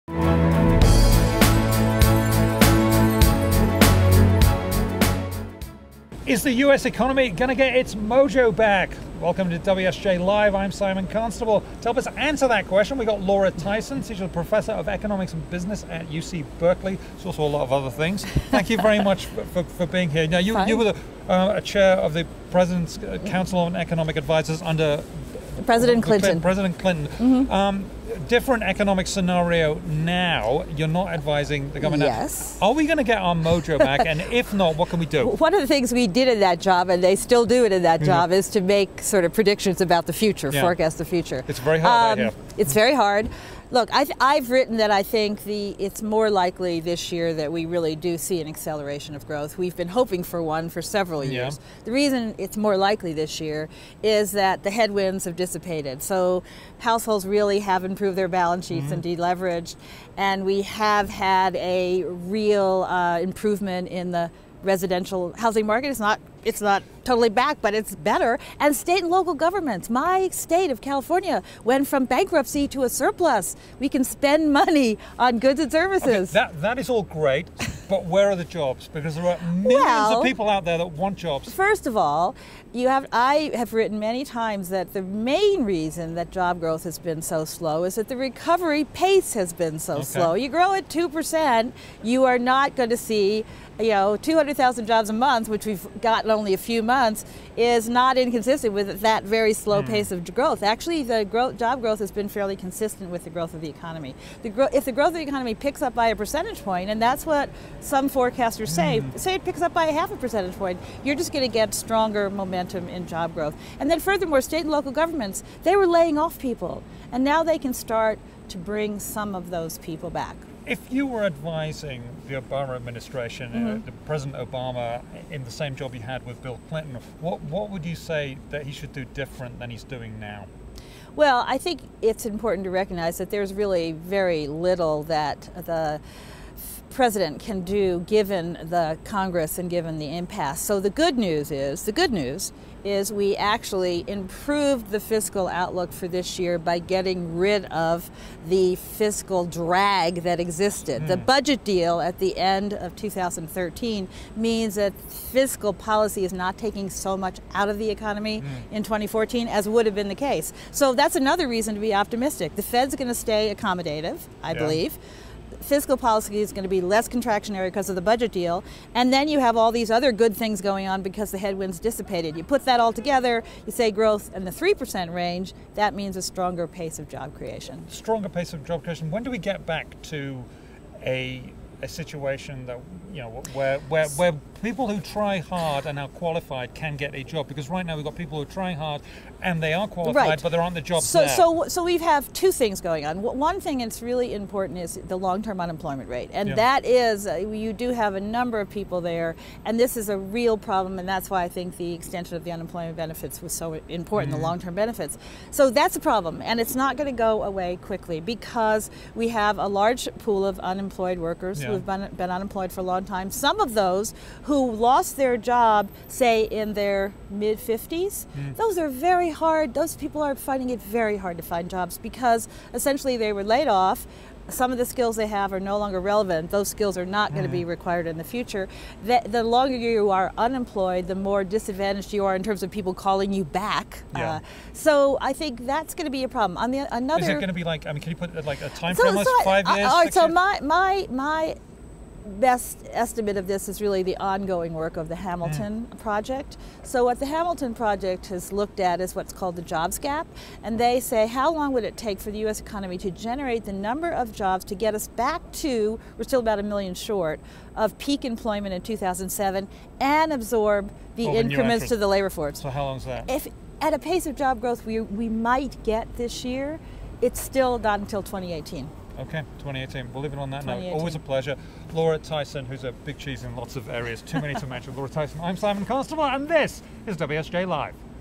Economic Outlook Looks Rosier Says Laura Tyson | Davos World Economic Forum
UC Berkeley Professor Laura Tyson at the World Economic Forum in Davos, Switzerland says the economic outlook for the U.S. is looking up. But it still may take years to claw back all the jobs needed to get back to pre-crisis levels. Photo: Getty Images